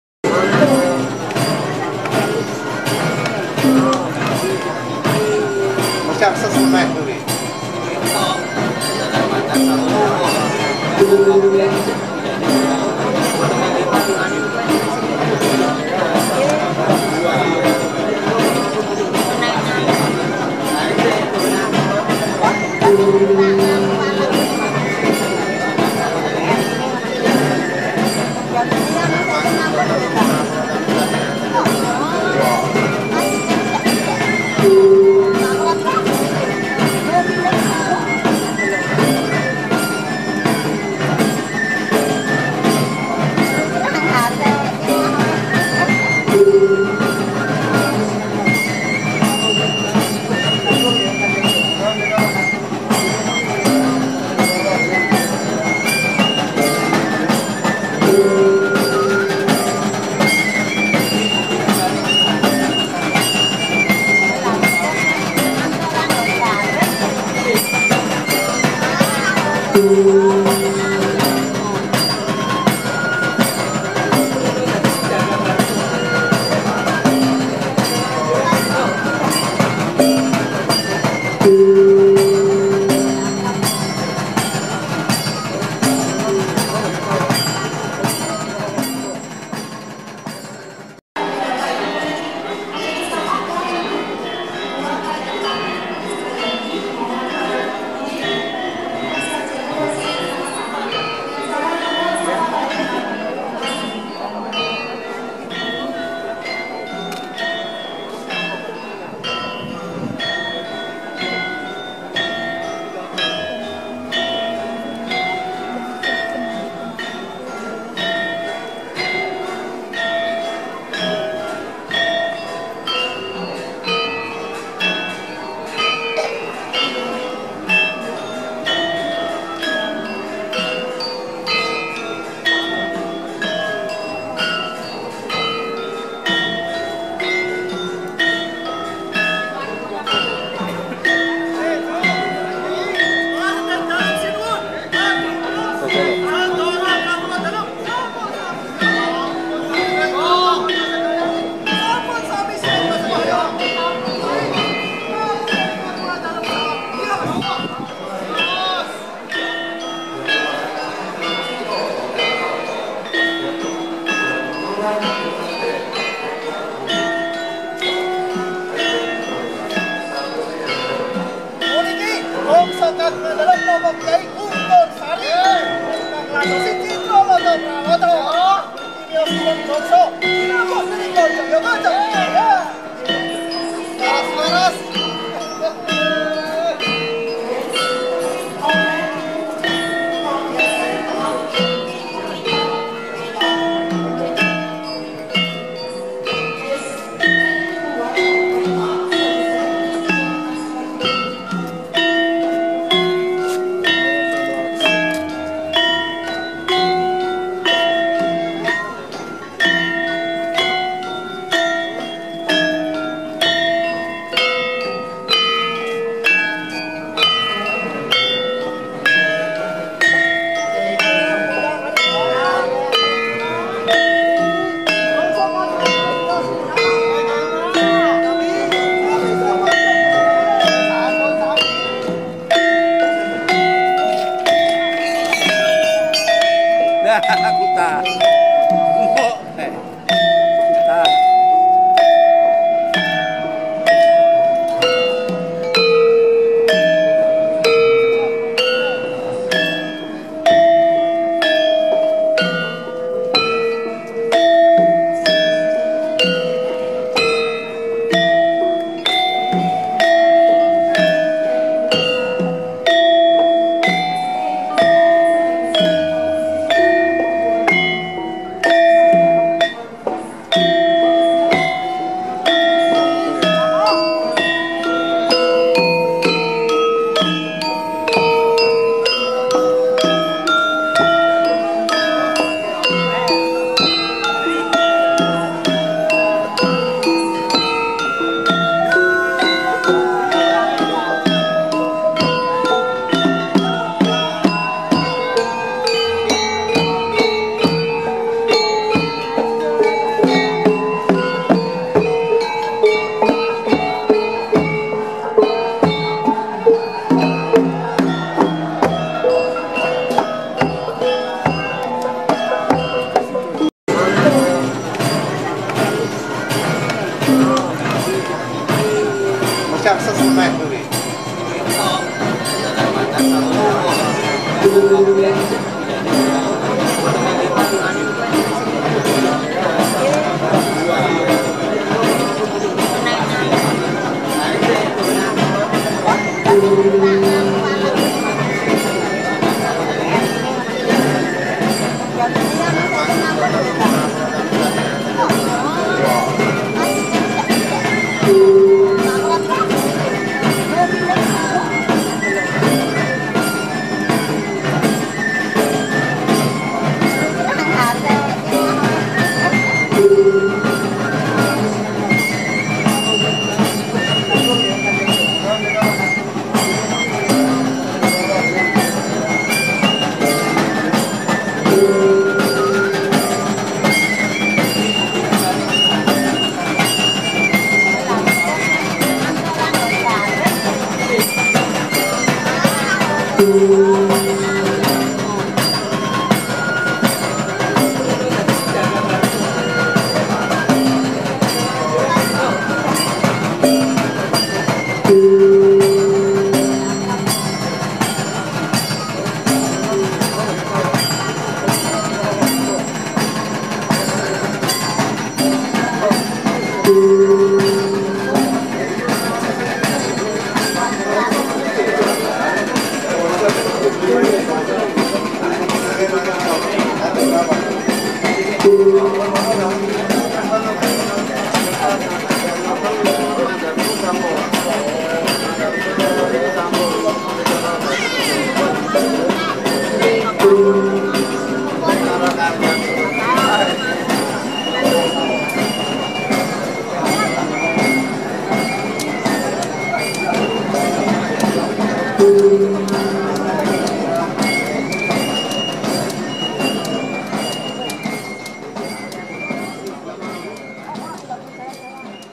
Perayaan Garebeg Mulud 2024 Keraton Yogyakarta
Garebeg Mulud 2024 diselenggarakan keraton Yogyakarta dengan defile Prajurit Keraton dan Gunungan untuk di bagikan ke masyarakat